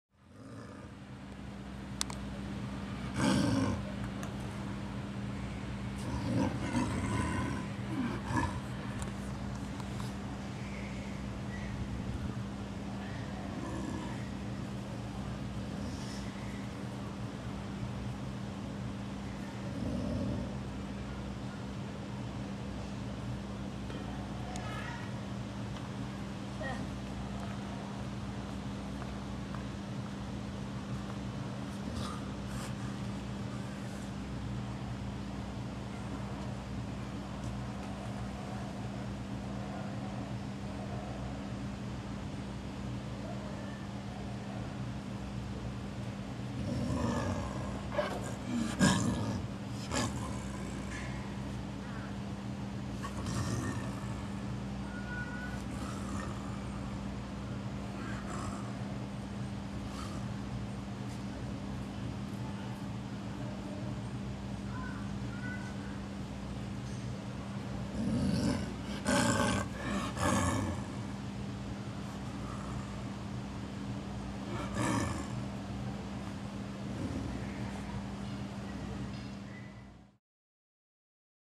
隣と交信中のユキヒョウ
円山動物園のユキヒョウ、アクバル(父)、ヤマト(オス)とユッコ(メス)。
隣のマス同士で暮らしていますが、時々、こうしたやりとりがあります。
ブログ「北の暮らし ~札幌・宮の森から~」内で、ユキヒョウ物語も毎日更新中。